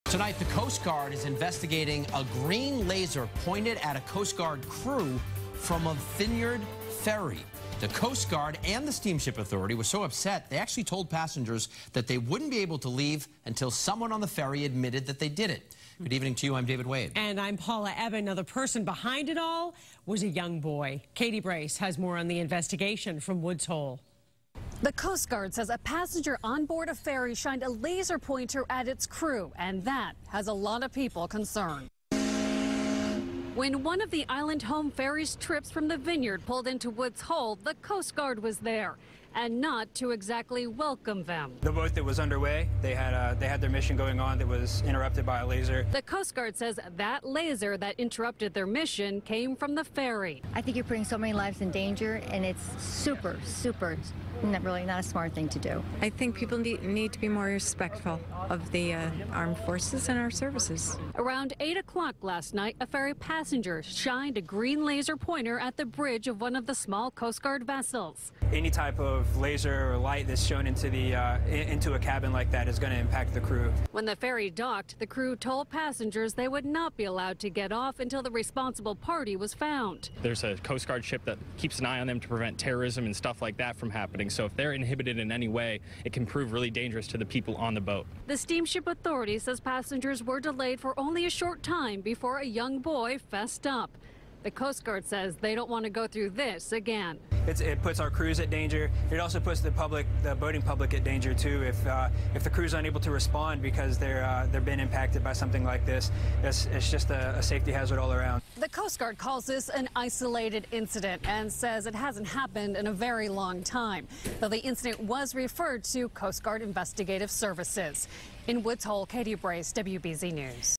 Young Ferry Passenger Interrupts Coast Guard Mission With Laser Pointer
WBZ-TV's Katie Brace reports.